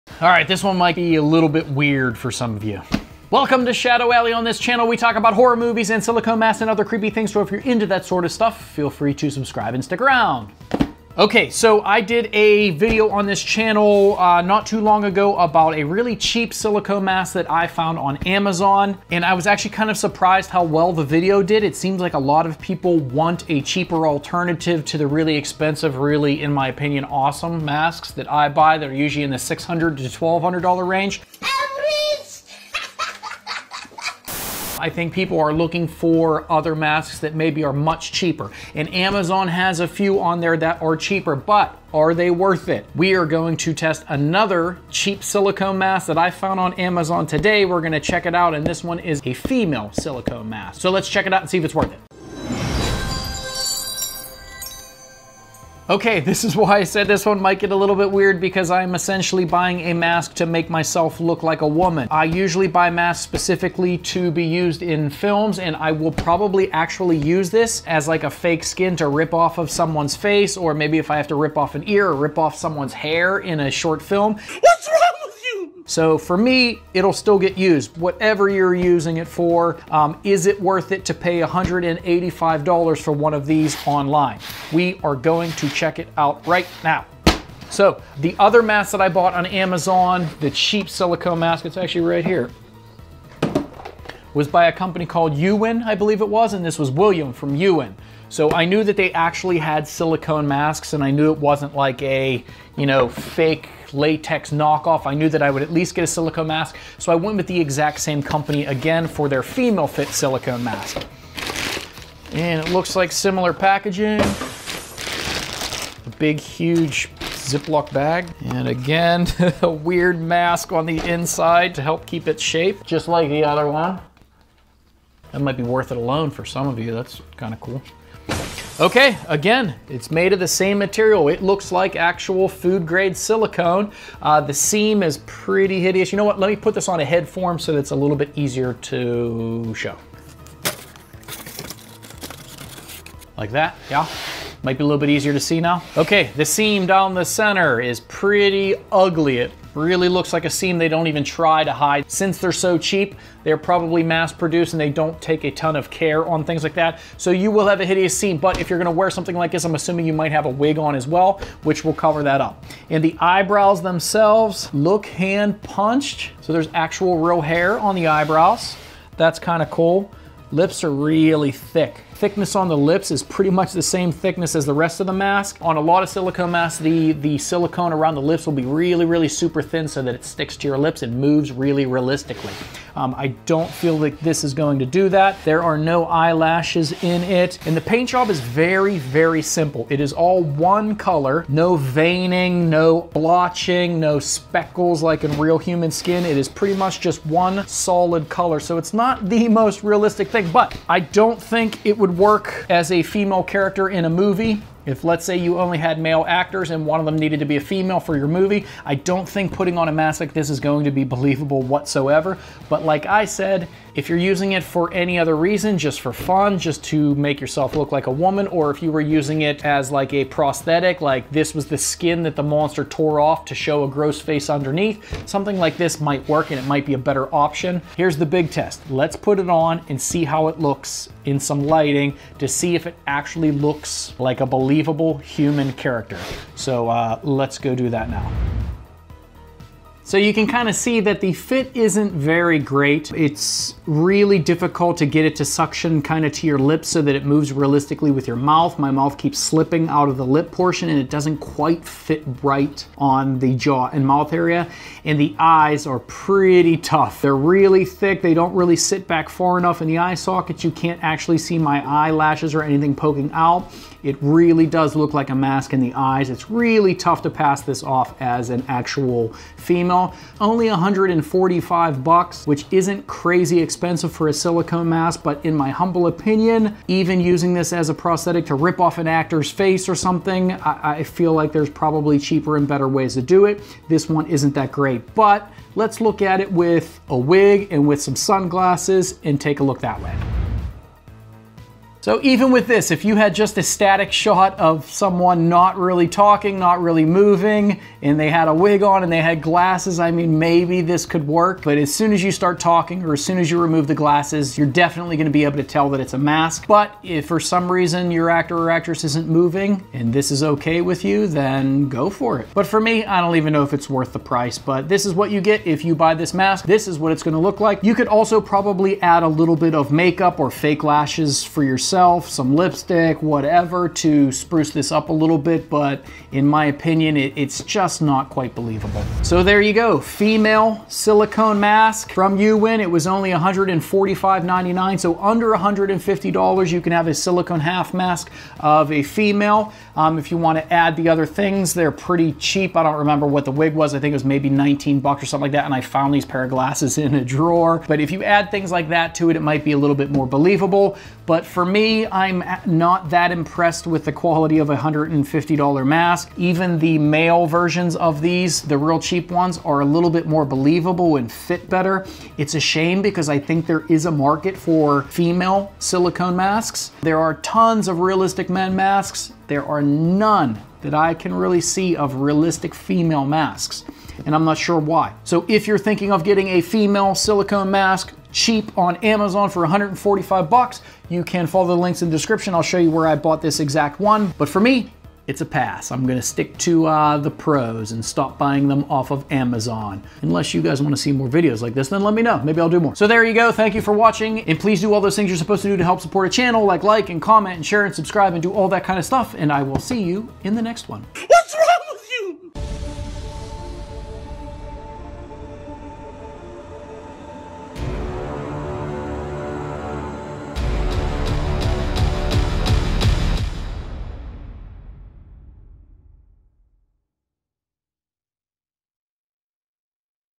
CHEAP Female Silicone Mask From Amazon - Is It Worth The Money? [Unboxing and Review]
Another CHEAP silicone mask from Amazon.. This time it's a FEMALE MASK!
Check them out on Amazon
The last cheap amazon mask review I did seemed to be well received so I thought it was time to do another one!  This is a cheap female silicone mask that I think is not quite worth the price.  It is much cheaper than the average silicone mask but it's far less realistic in my opinion.

Some Of My Favorite Equipment
Here's where I look for used (cheaper) masks:

Let me know your thoughts on this female fit silicone mask review... What did you like/hate?.. Was it realistic enough?